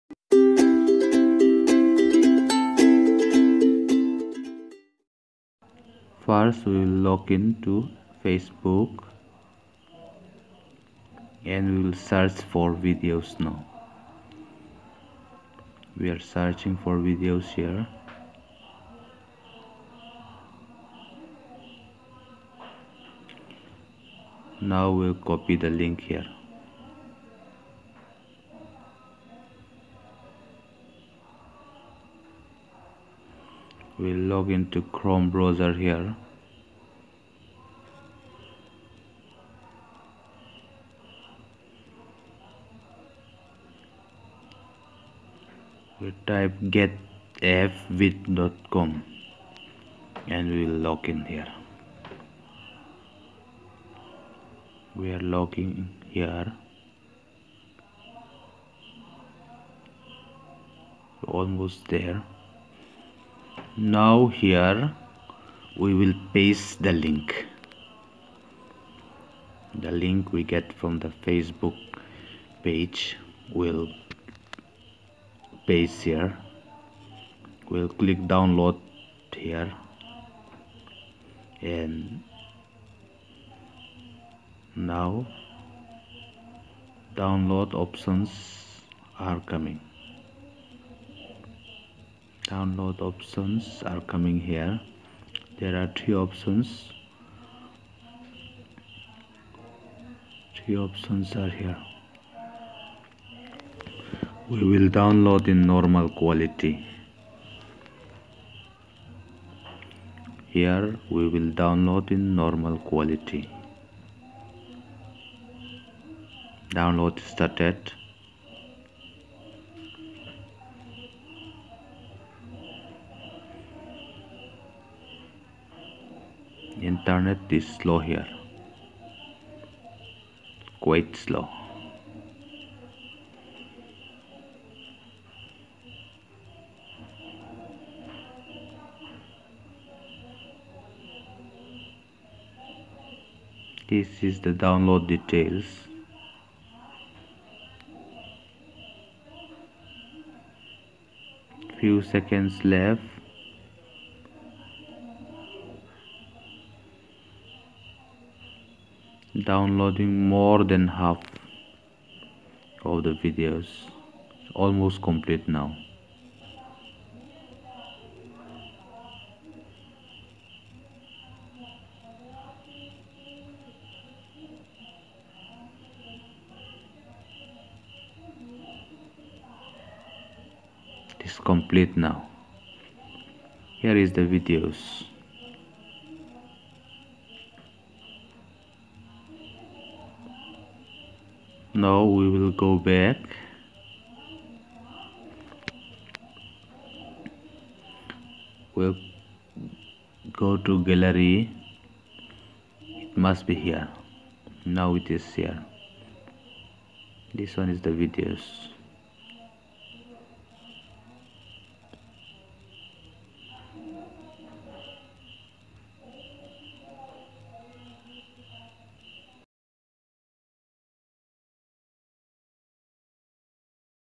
Download facebook videos easy way without any Apps
Download fb videos with ease and comfort from your mobile